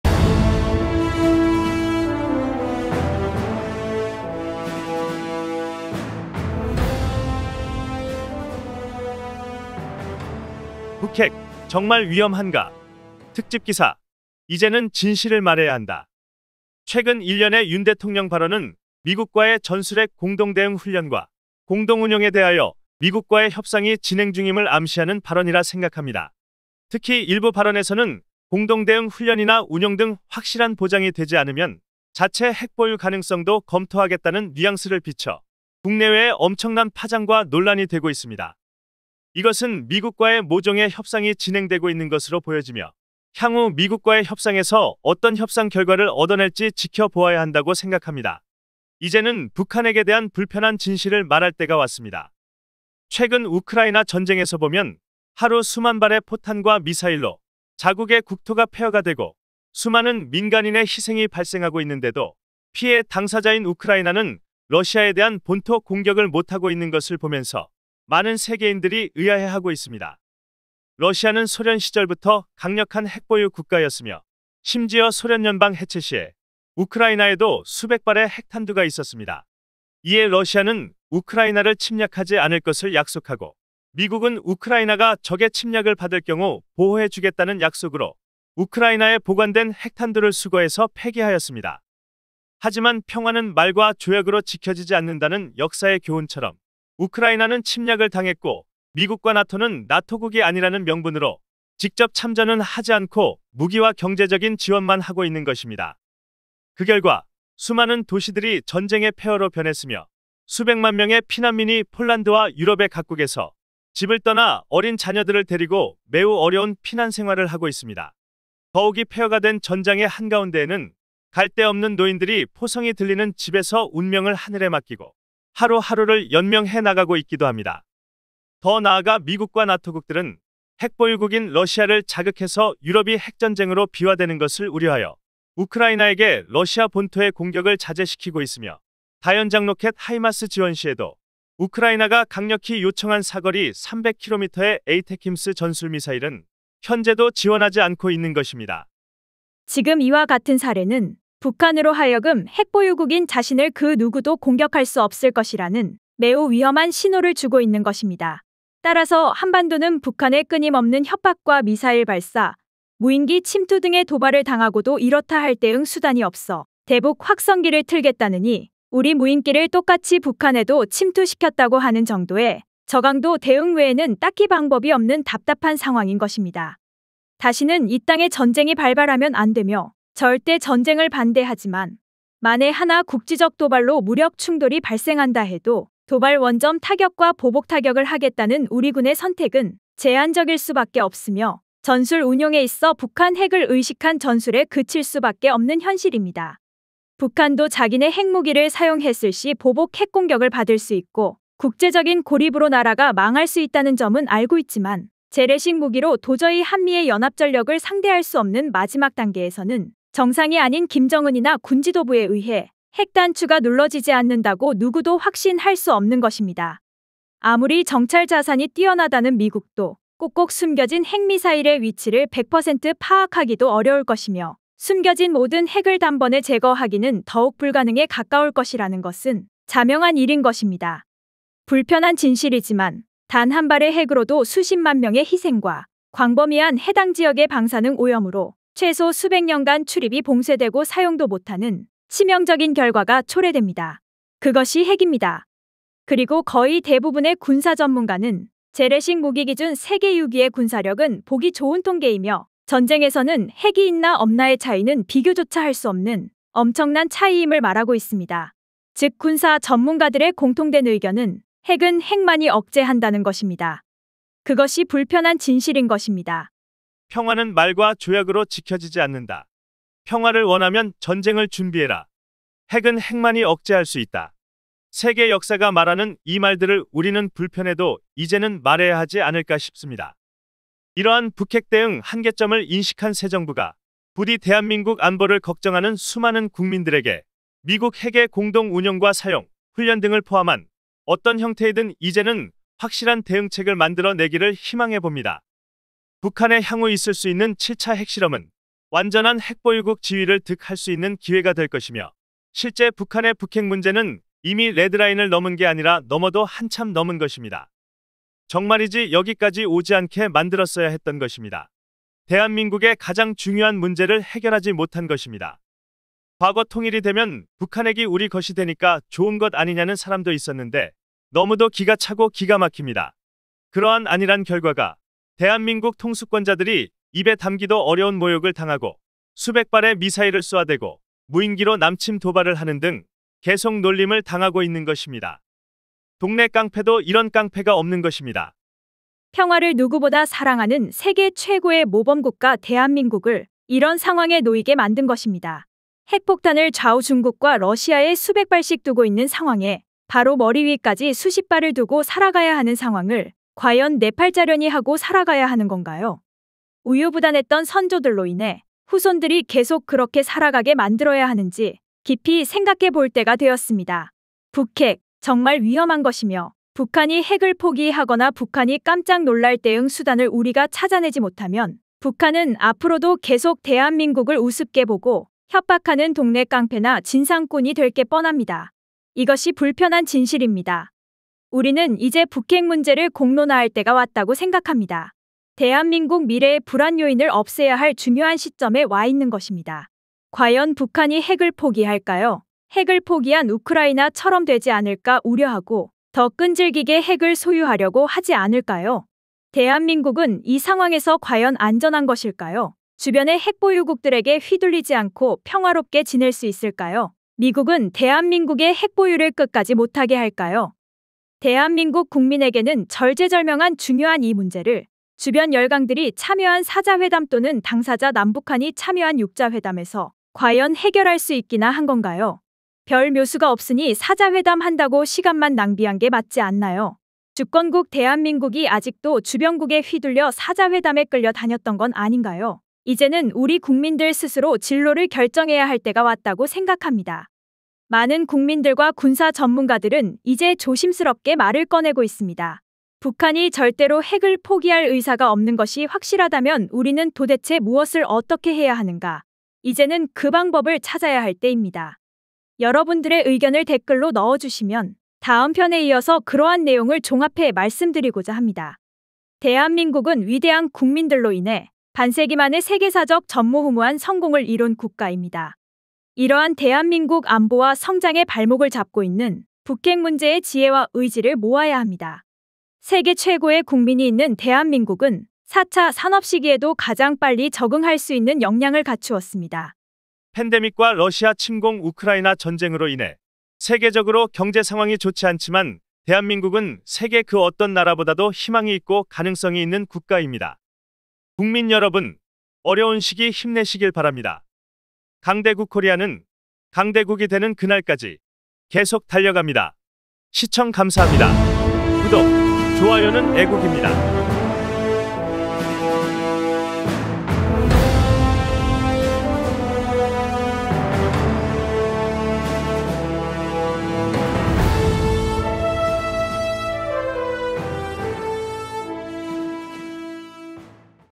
[특별기획] " 핵은 핵만이 막을 수 있다. " 불편한 진실
대한민국 안보를 걱정하는 수많은 국민들에게 미국 핵의 공동 운용과 사용, 훈련 등을 포함한 어떤 형태이든 이제는 확실한 대응책을 만들어 내기를 희망합니다.

북한의 향후 있을 수 있는 7차 핵실험은 완전한 핵보유국 지위를 득할 수 있는 기회가 될 것이며 실제 북한의 북핵 문제는 이미 레드라인을 넘어도 한참 넘은 것입니다.

정말이지 여기까지 오게 만들지 말았어야 했던 것입니다.
대한민국의 가장 중요한 문제를 해결하지 못한 것입니다.
이제는 분명한 대책을 마련해야 합니다.

강대국이 되는 그날까지.. 강대국 코리아는 달려갑니다.!

대한민국 애국 채널..

강대국코리아TV